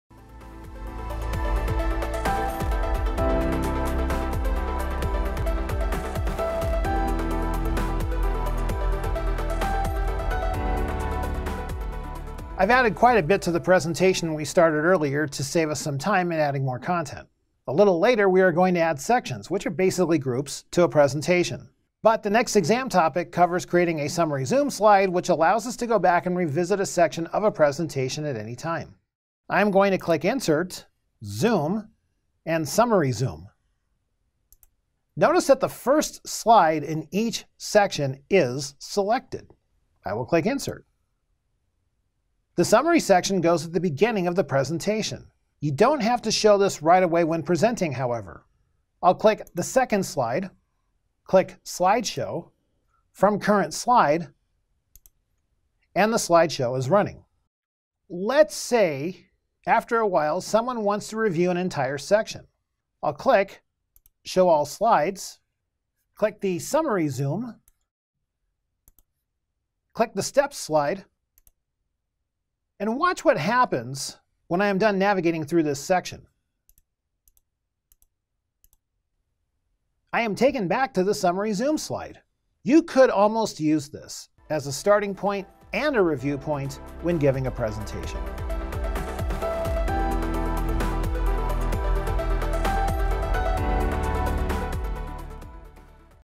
Insert Summary Zoom Slides - PowerPoint 2019 tutorial
In this video, LearnKey expert Jason Manibog demonstrates how to create a slide with a Summary Zoom in a PowerPoint presentation.